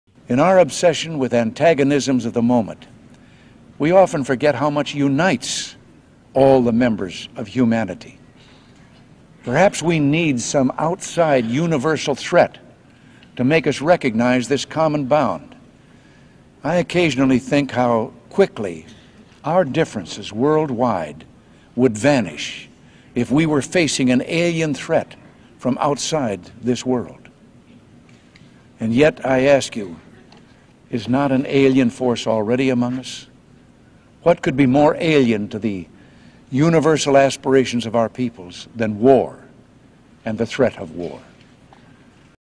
All US Presidents are Psy-Ops
President Ronald Reagan on an Alien Threat at the United Nations - 1987.

What better way to usher in the final stages of the NWO than an 'Invisible enemy' that doesn't discriminate against nation and ethnicity.
- We are ALL victims
- We are ALL required to fight the war
- We are ALL going to be enslaved by the Cabalist Elite's.